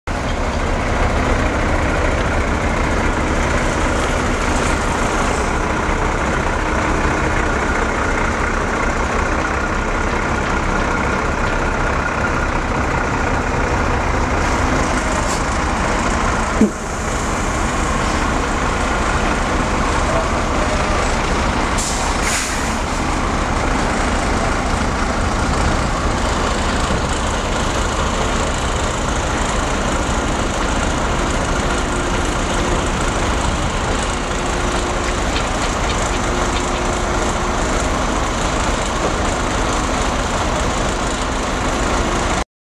22.07.2013.
Demontaža krana na gradilištu u Bloku 61-Novi Beograd-NEIMARv